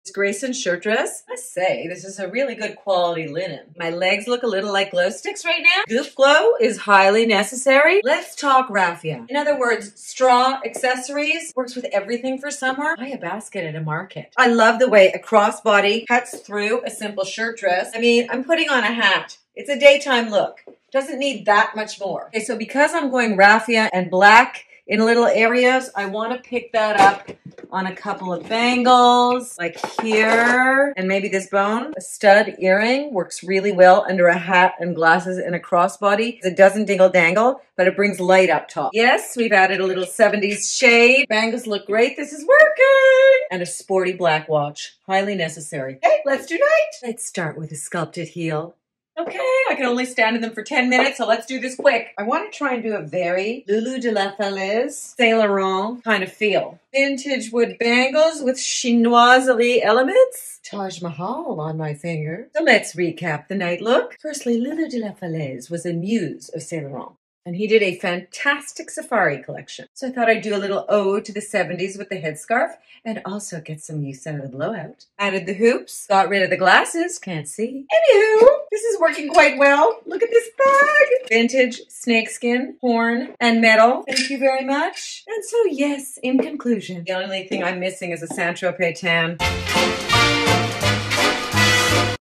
Double Duty: One Dress, Two Looks | Linen Shirt Dress | Over Fifty Fashion | Carla Rockmore
Double duty: One dress, two completely different looks. Thats when you know a it’s a keeper. This Grayson linen is really great quality, too!

for $25 off your purchase!